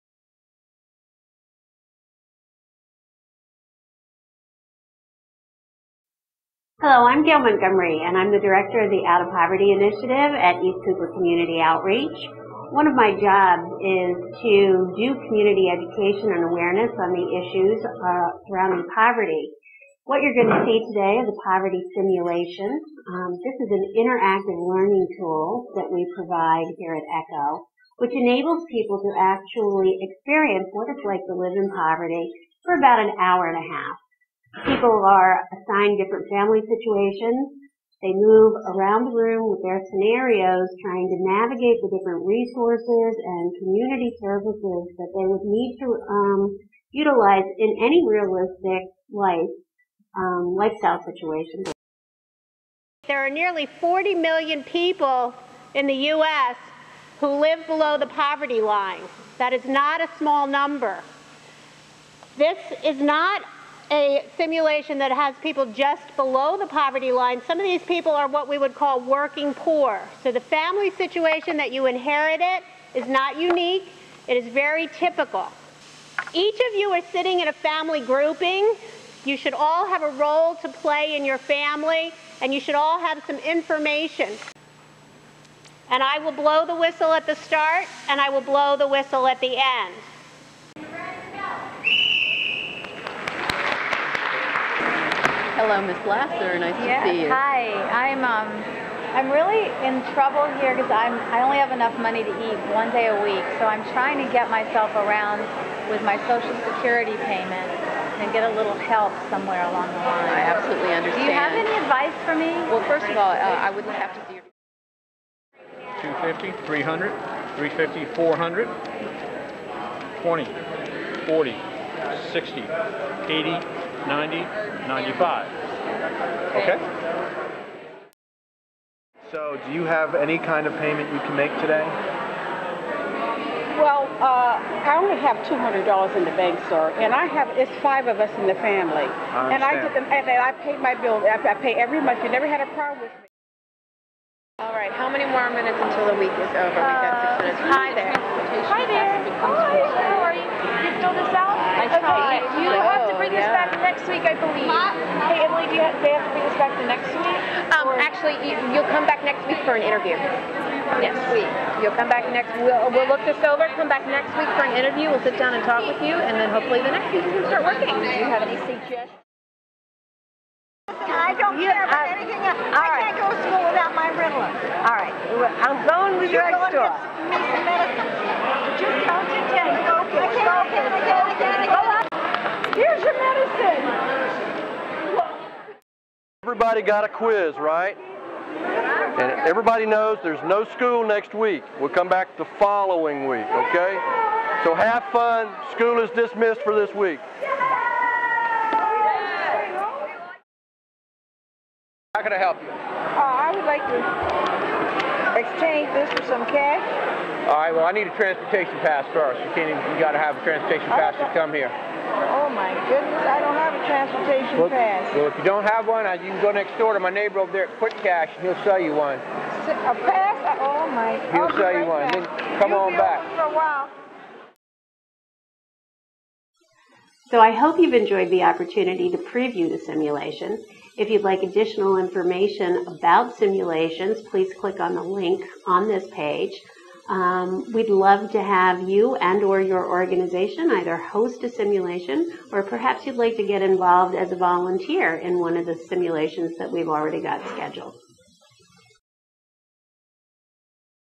ECCO Out of Poverty Simulation
ECCO's Out of Poverty Initiative is a multi-faceted effort inspired by Ruby Payne, and her groundbreaking work of helping people understand those in poverty. The program is designed to engage everyone in the East Cooper area in the effort to eliminate poverty and make our community a better place to live.